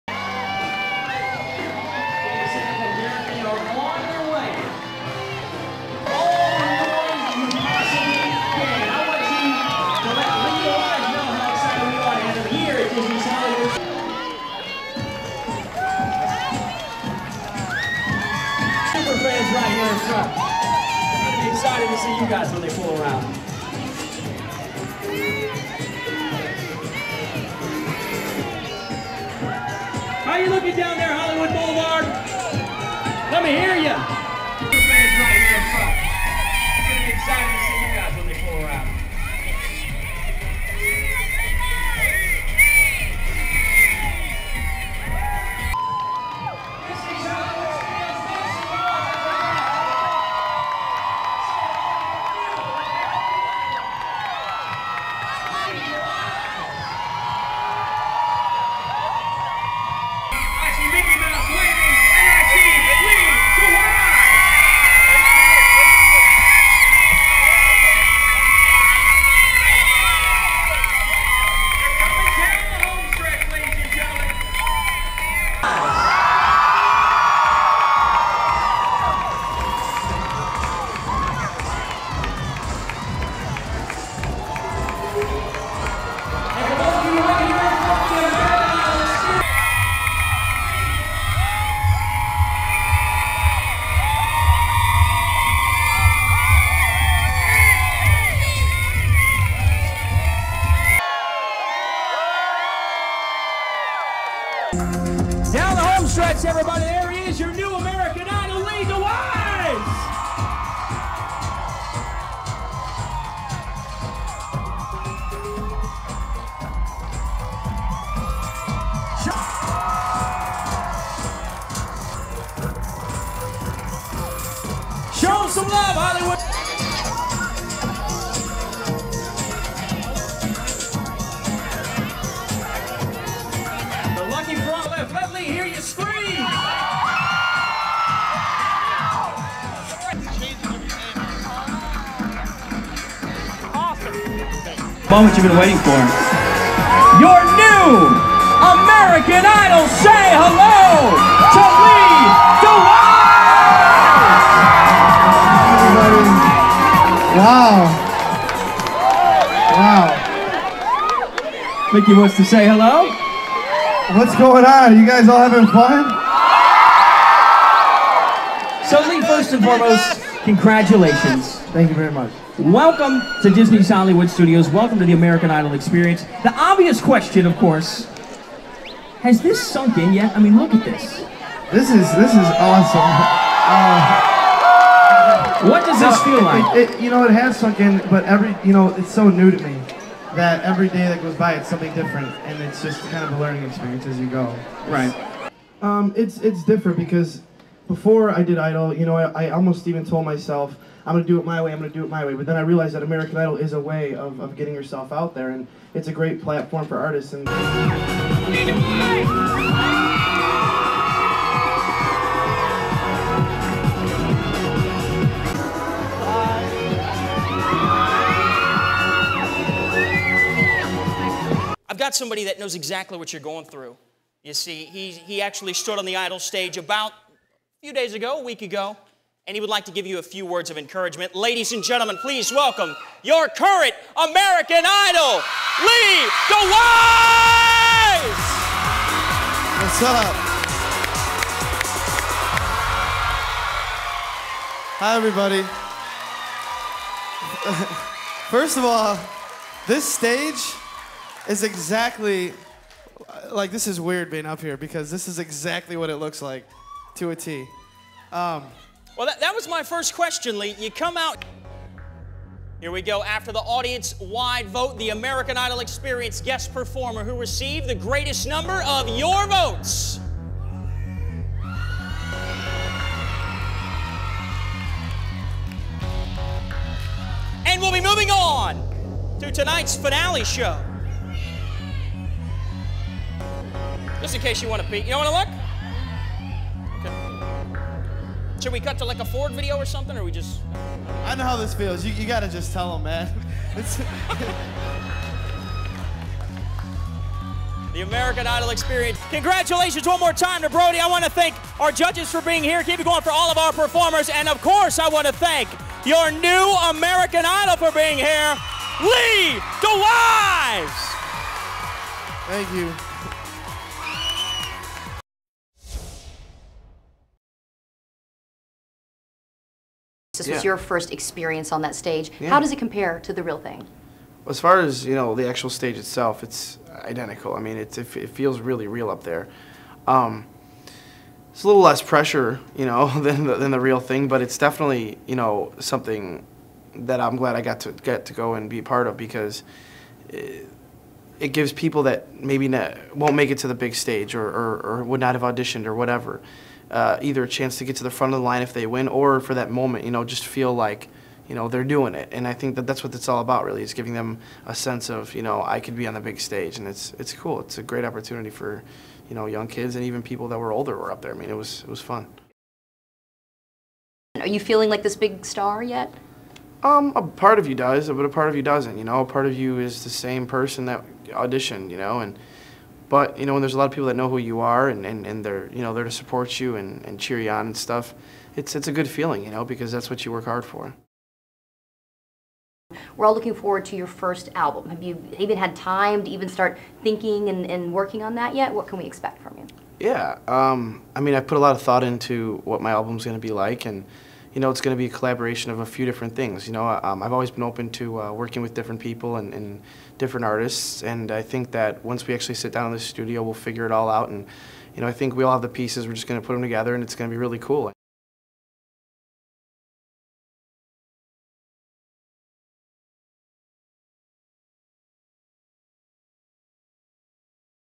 American Idol Winner Lee DeWyze visits Walt Disney World
courtesy of Disney